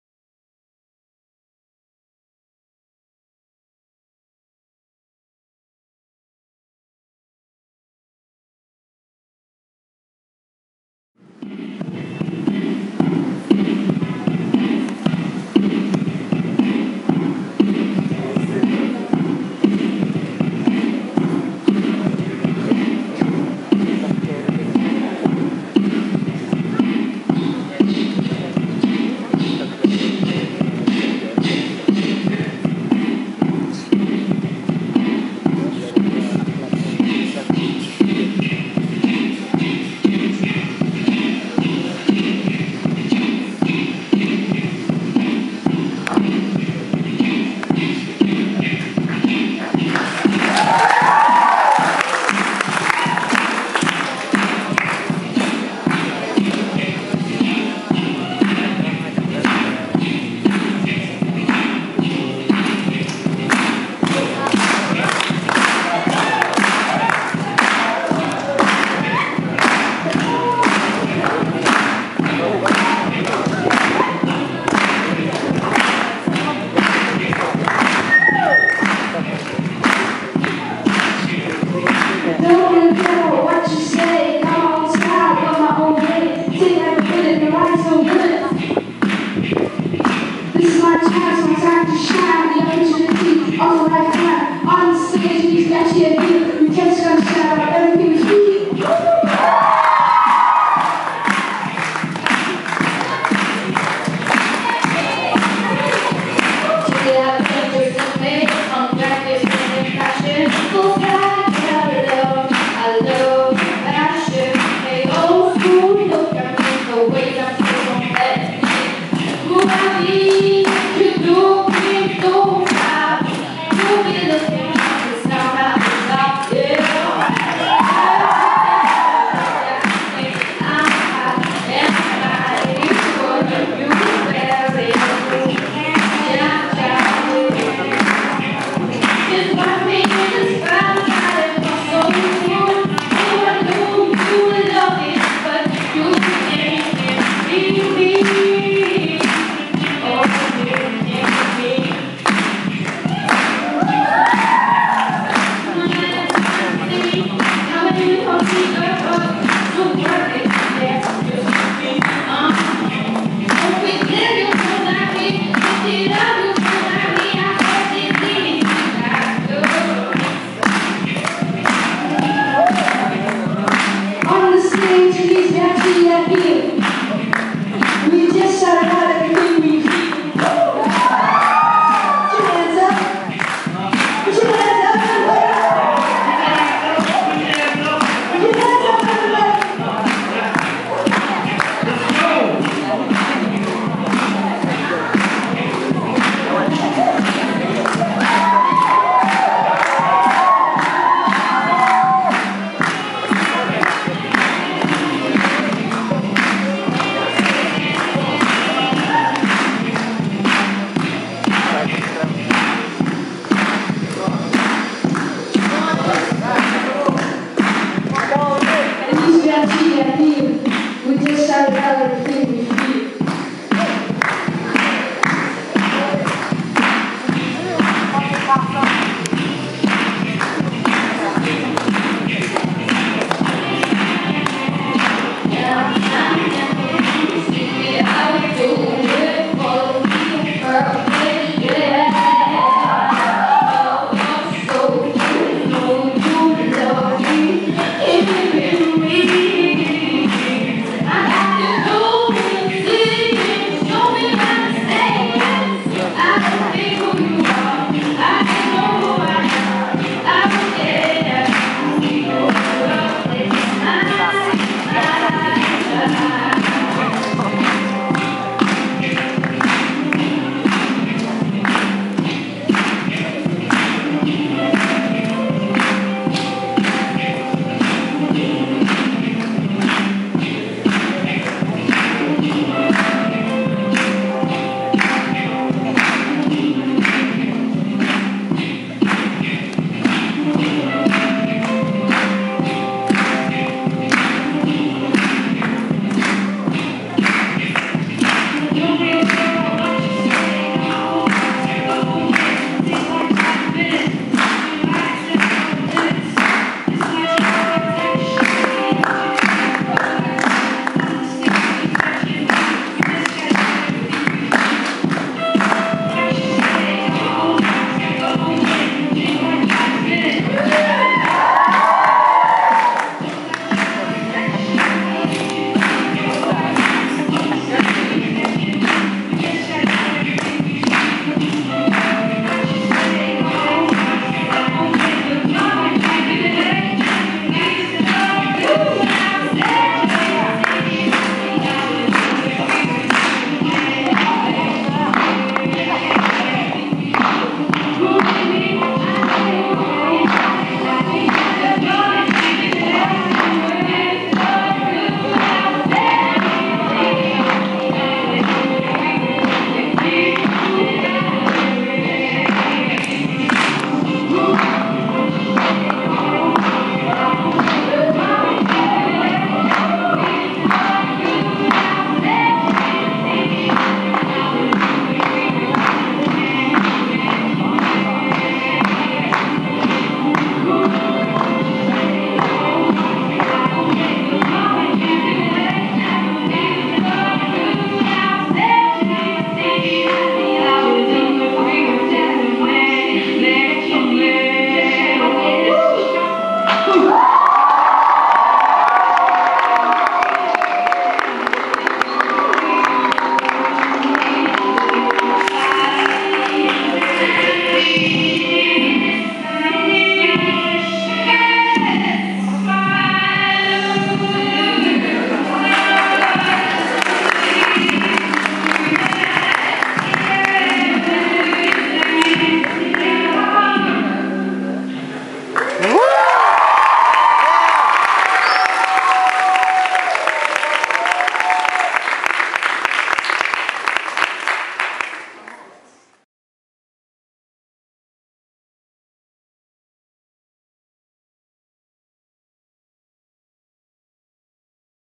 Solochiman & KC Secondary School - a live looping experience
A live looping experience with Solochiman & the students from KC Secondary School of shillong, meghalaya, india
27 july 2013

Live looping is a composition technique for recording and layering  sounds in real time, so we able to create and build a song in the same time we are playing and/or singing,
 it can be used to create any style of music.

Thanks to:
ayesha langstieh ( producer assistant)
gino thangkhiew  ( audience helper)
david livingston   (video recording)
brian wallang       (KC School director)